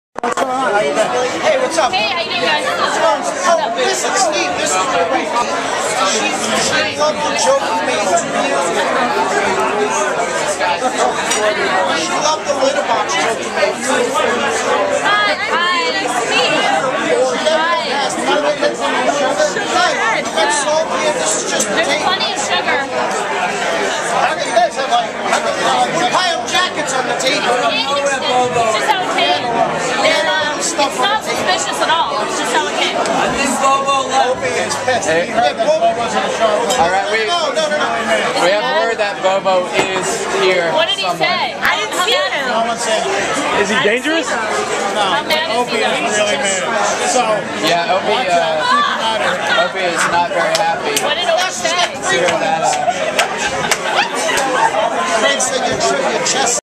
Bobo Security Searching For The FH Fugitive
Recorded on December 10, 2011 using a Flip Video camera at FH Riley's. Security came over to investigate reports that Bobo had snuck into the restaurant uninvited.